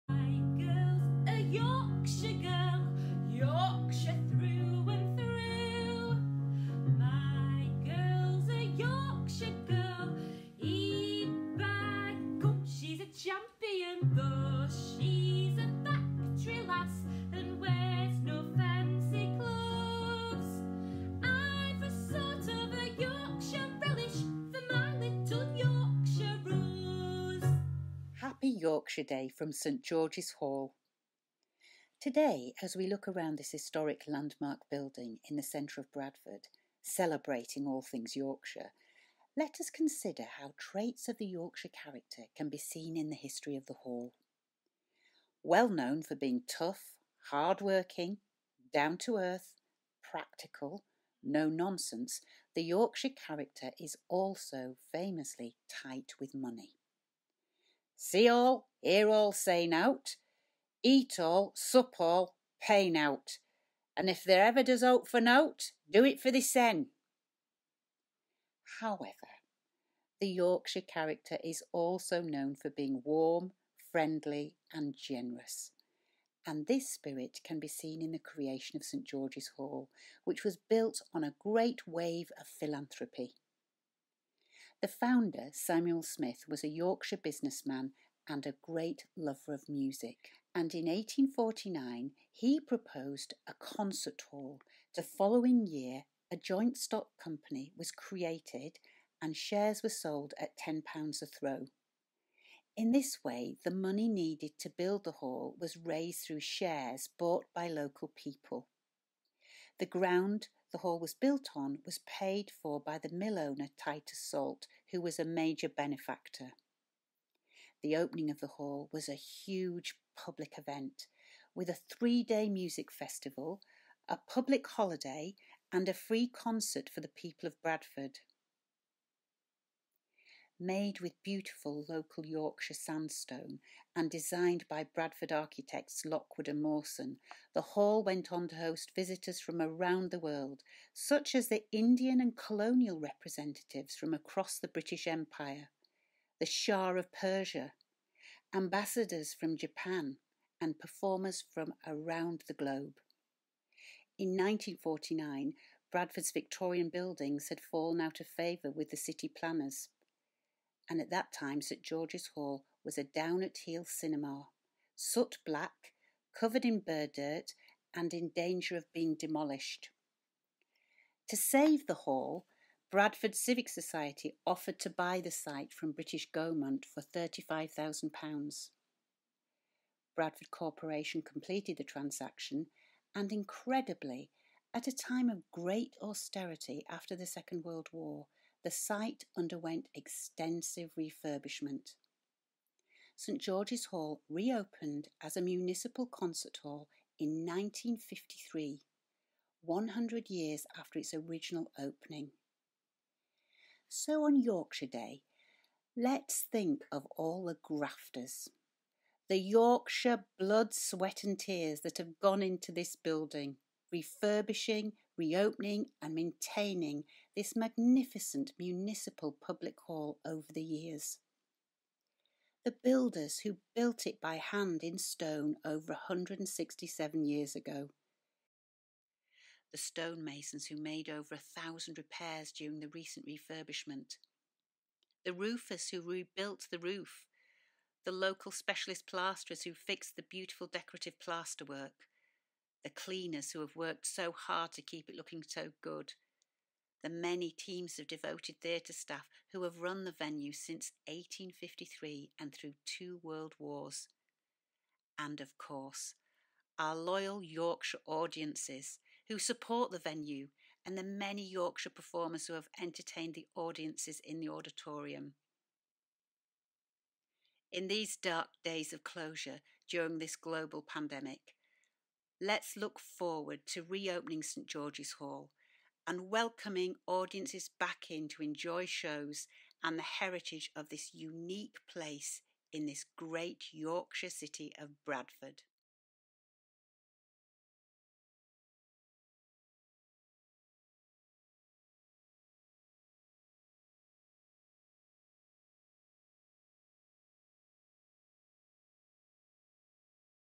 Yorkshire Day 2020 St George's Hall
A look at this beautiful landmark building as we celebrate all things Yorkshire.